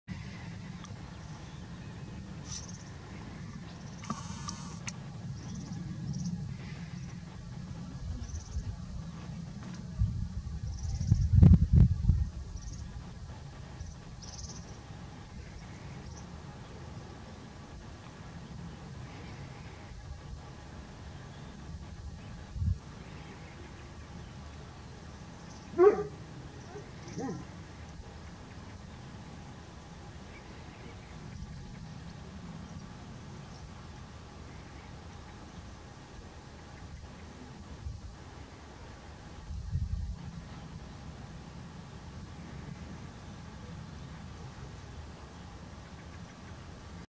Milano Real (Milvus Milvus). Quijorna.
Milano Real intentando robar una presa a Cigüeña Blanca.
Quijorna (Madrid). Spain. 03/2017.